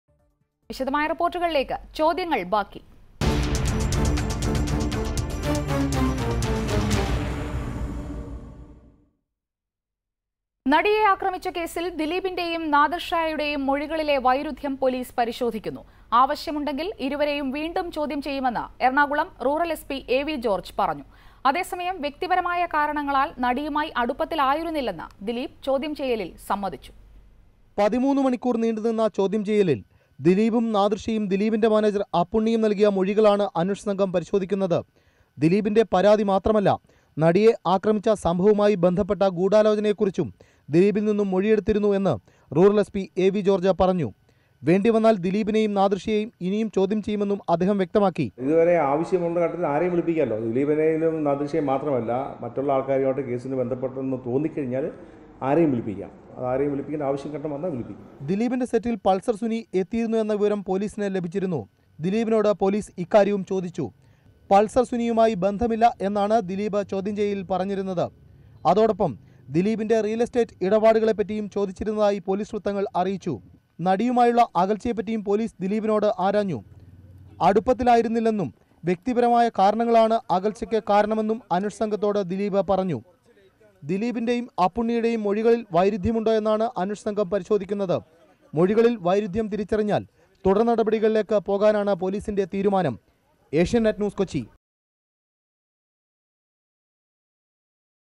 Actress attack case: No cordial relations with actress, says Dileep
were differences of opinion with the actress, who was abducted and attacked in Kochi says dileep